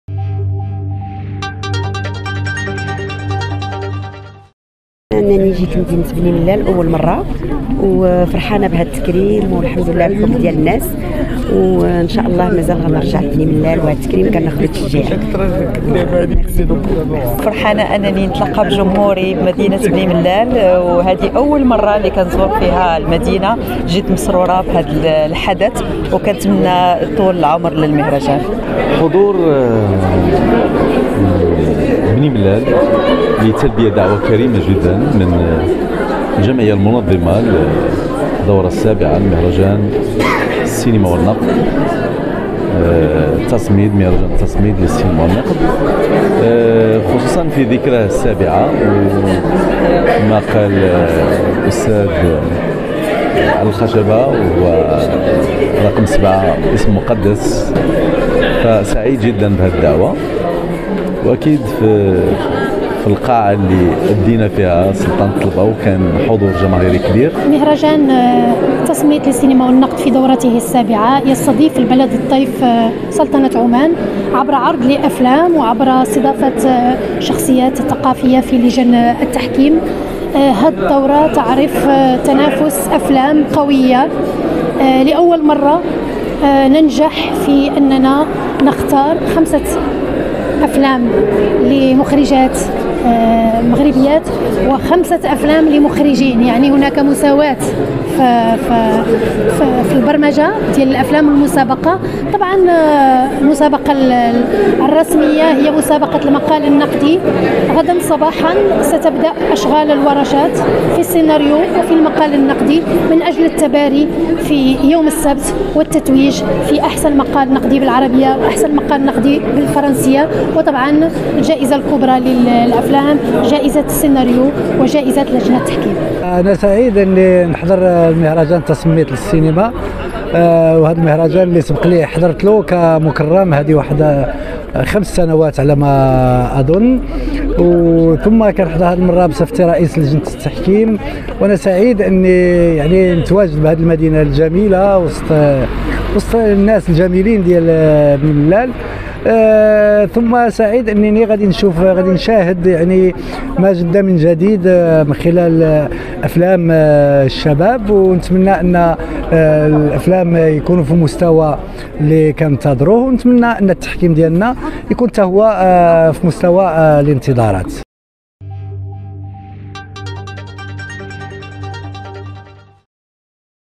الأنيقة ماجدولين الإدريسي والجميلة نجاة الوافي والفنان هشام بهلول وآخرين حاضرون في مهرجان تاصميت
الأنيقة ماجدولين الإدريسي والجميلة نجاة الوافي والفنان هشام بهلول وآخرين حاضرون في مهرجان تاصميت ببني ملال.